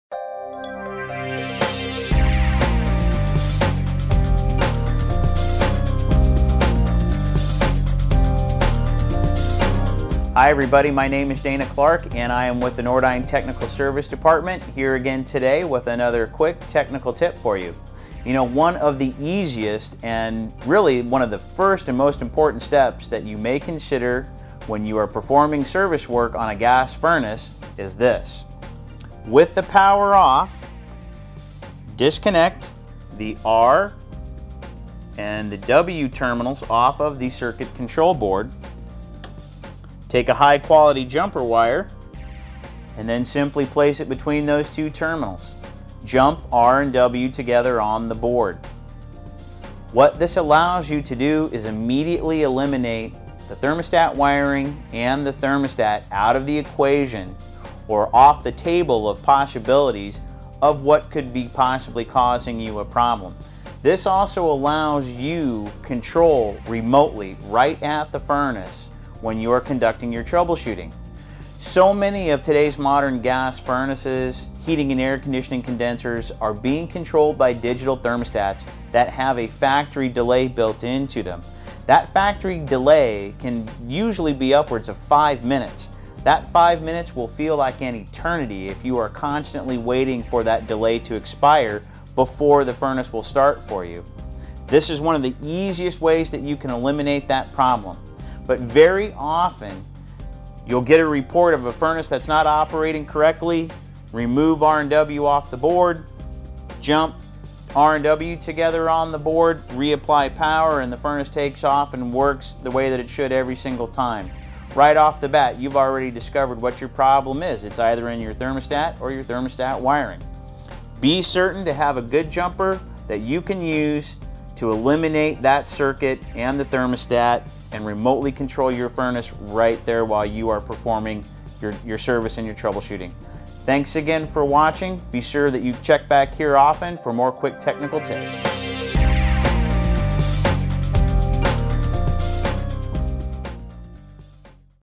Furnace Repair - Eliminate the Thermostat | NORDYNE U Technical Tip
Furnace repair can be made easier by eliminating the thermostat before you start working. This takes one circuit out of the equation and allows you to concentrate on other relevant areas.